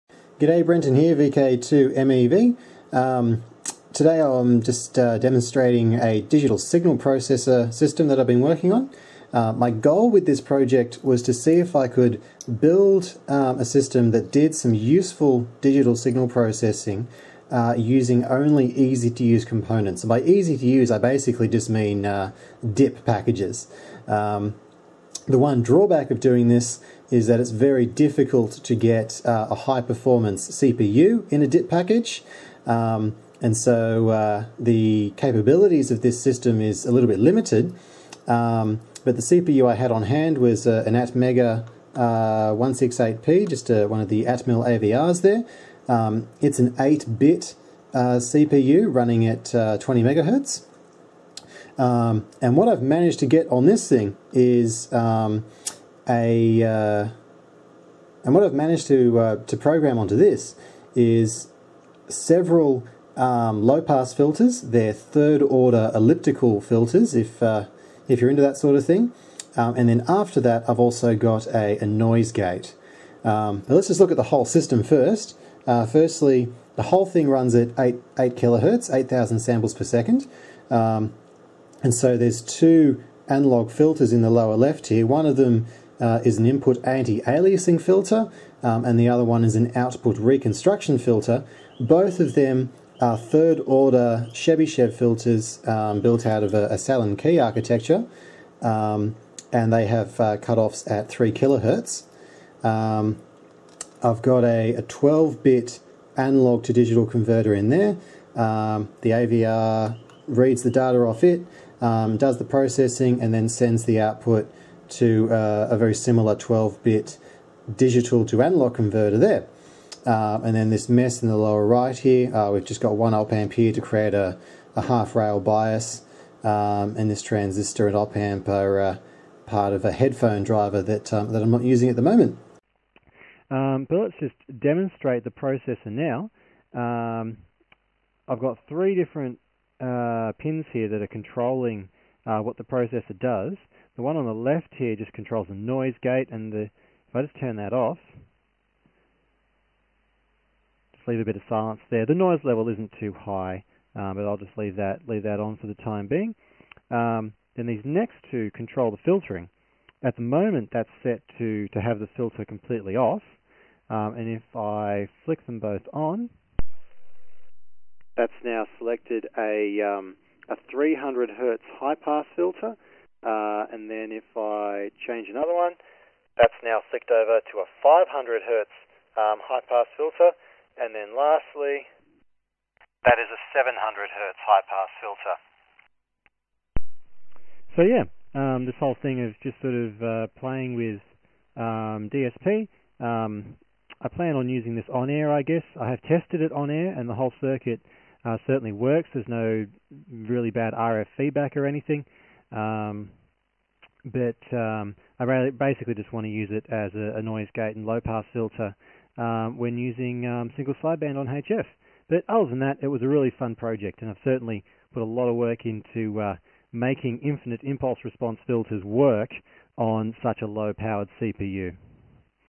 Cheap Homebrew DSP Demo
This project was an experiment in using a general purpose microcontroller (ATMEGA168P) to perform digital signal processing. Such a chip will never have high performance, but I wanted to know how much could be done with one.

Final performance was a single 3rd order IIR filter plus noise gating, this used about 75% of the 2500 instructions available to each sample.

Total parts cost (AVR, ADC, DAC) is about $15 (single unit quantities), which is peanuts for a DSP system. Furthermore, all the parts are easy to obtain (Element14, RS etc) and come in DIP packages, making homebrew experimenting very easy.